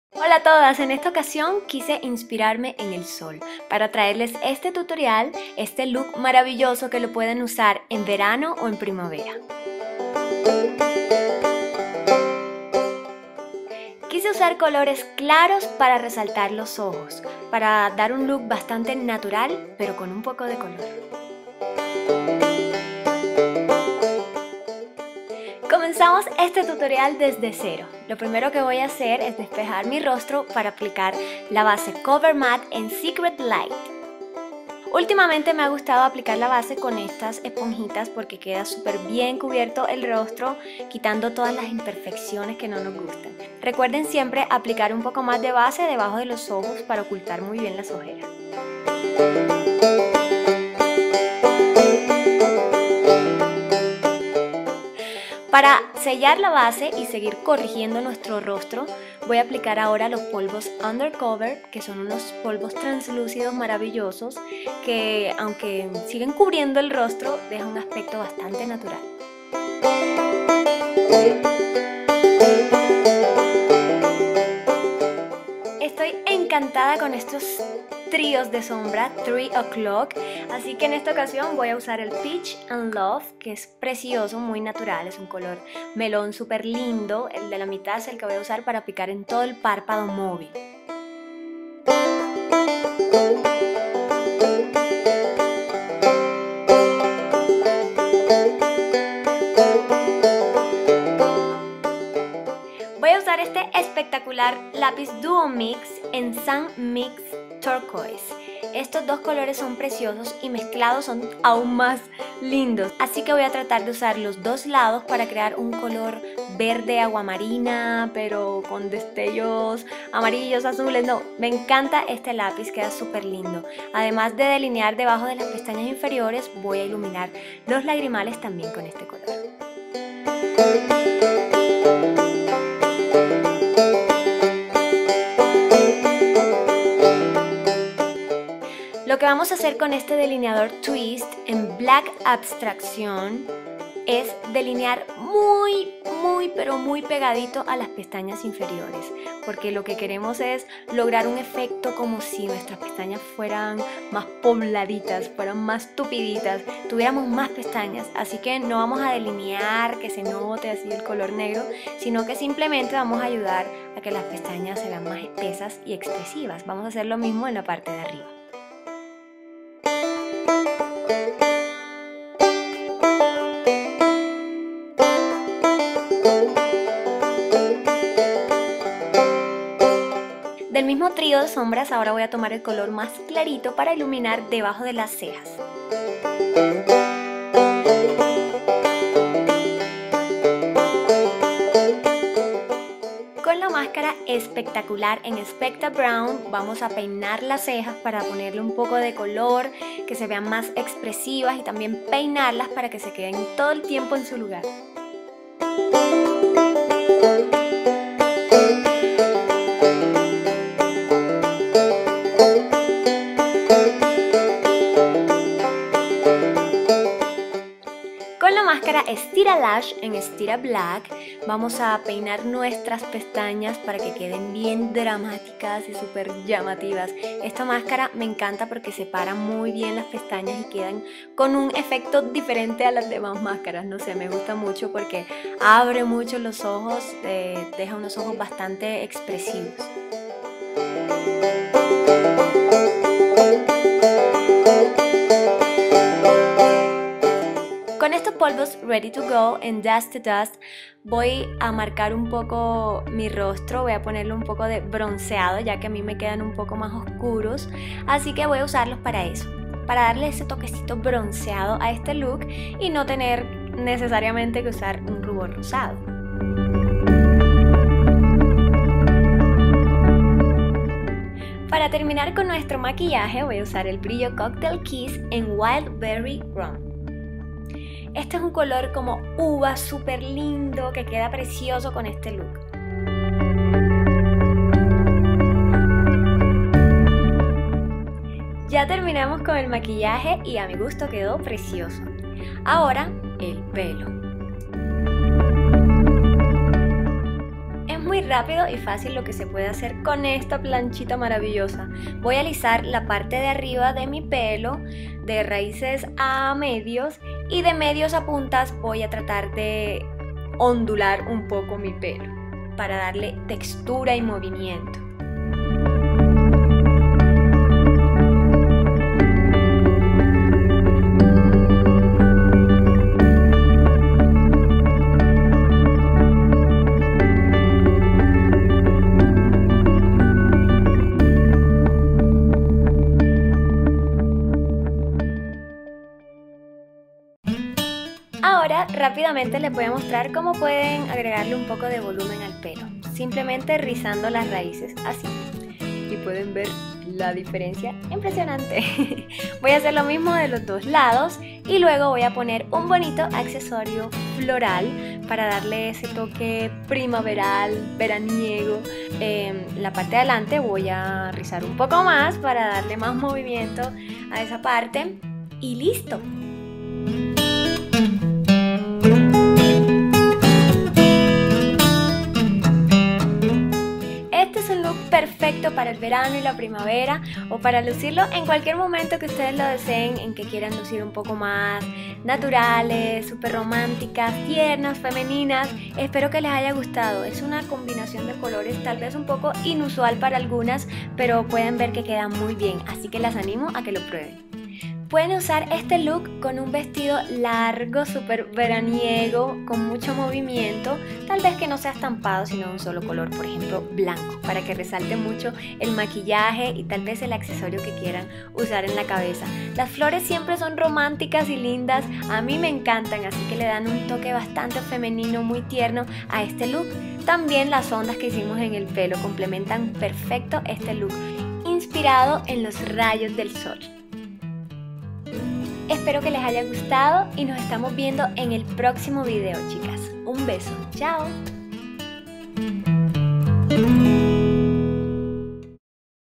Cy Make Up Tutorials - Sunlight Make Up
Martina nos enseña a armar este look natural, pero con un poco de color. Ideal para usar en primavera o verano! Estos son los productos que utilizó:

Base Cover Mat, tono Secret Light
Polvos translúcidos Undercover
Sombras Three O'Clock, tonos Peach & Love:
Polvos Ready to Go, tono Dust to Dust
Brillo Coctel Kiss, tono Wild Berry Rum: